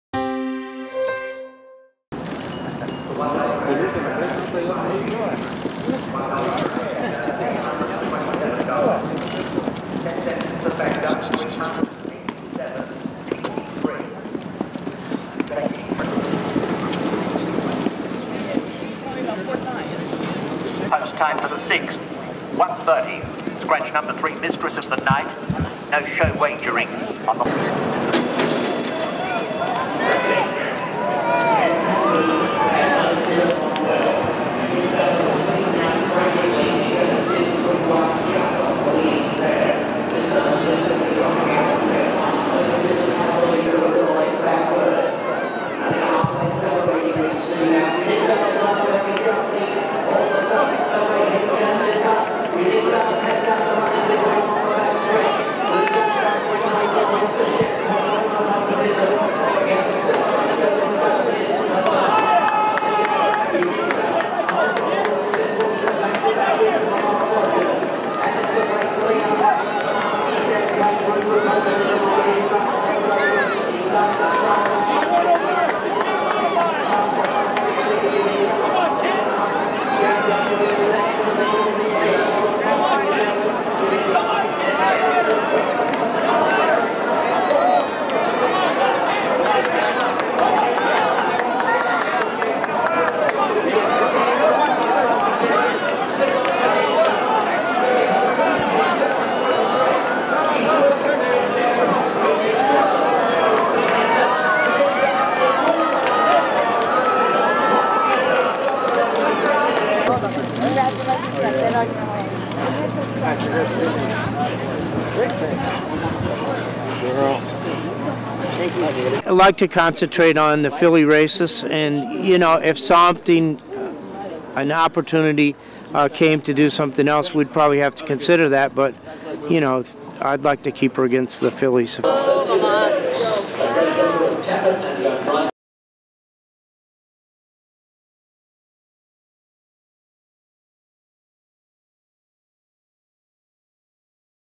Unique Bella is racing's newest star
3-year-old filly races to 8 3/4-lengths win in Las Virgenes Stakes